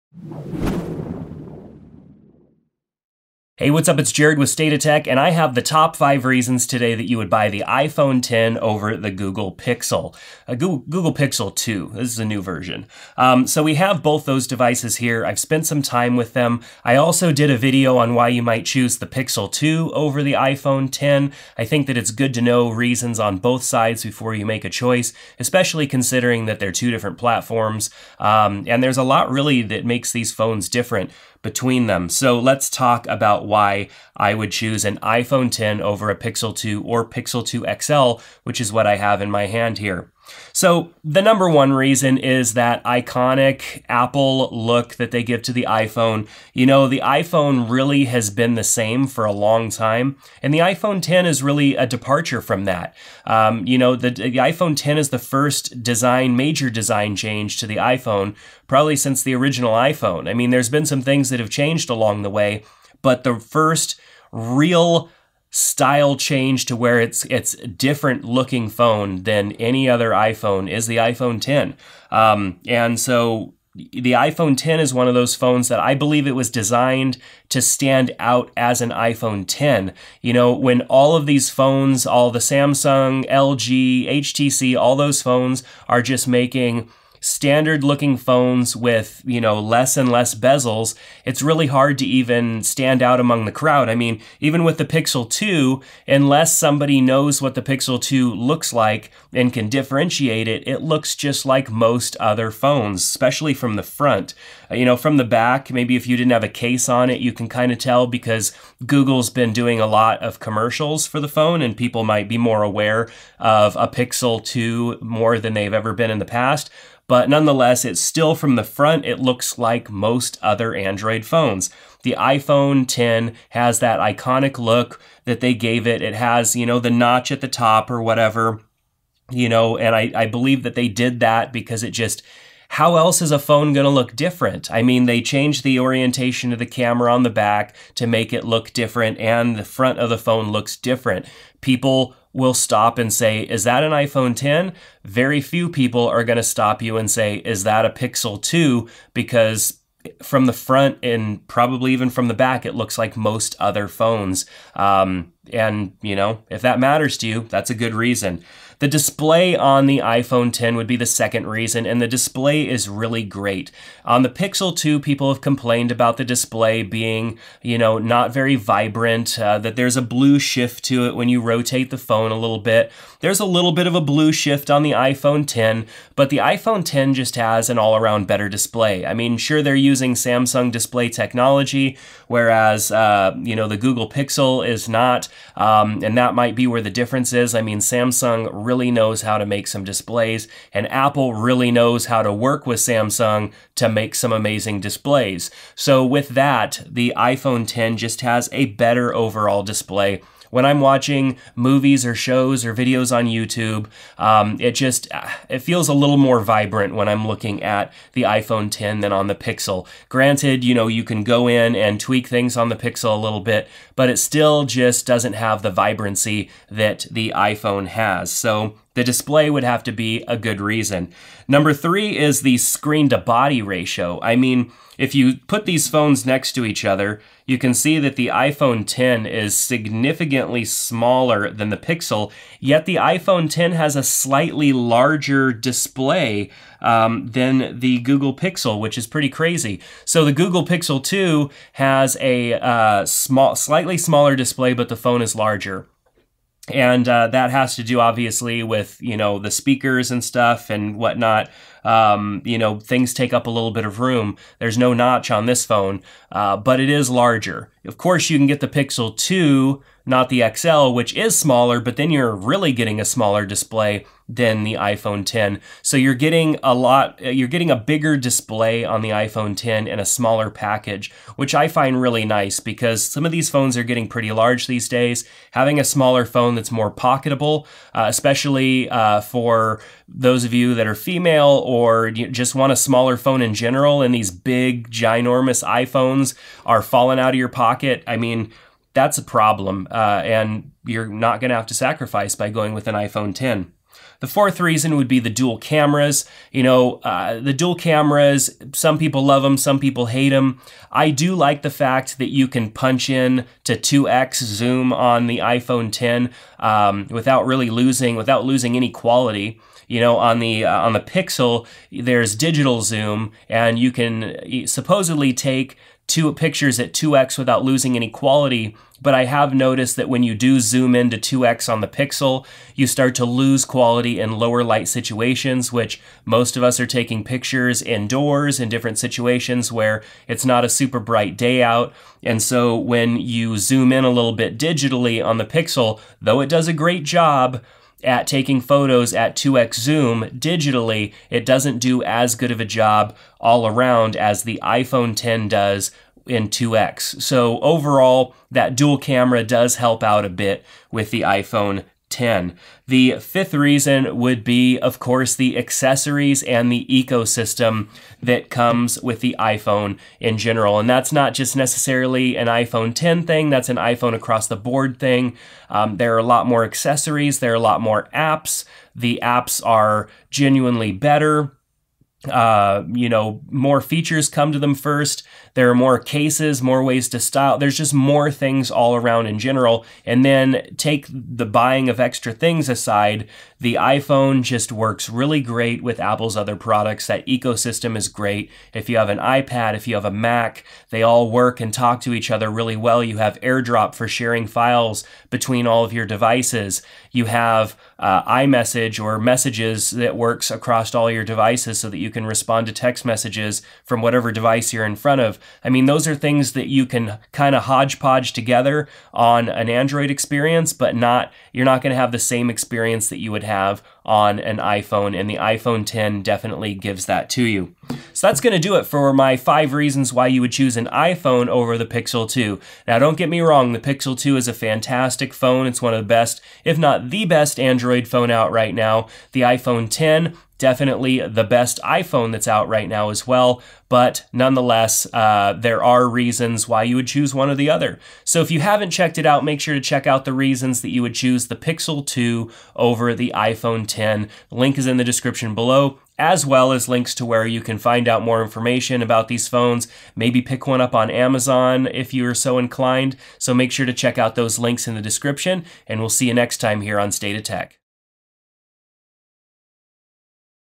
Top 5 Reasons to Choose the iPhone X Over the Pixel 2
You've seen the commercials and are probably wondering what makes the iPhone X better than the Google Pixel 2. In this video I discuss five reasons to choose the iPhone X over the Google Pixel 2.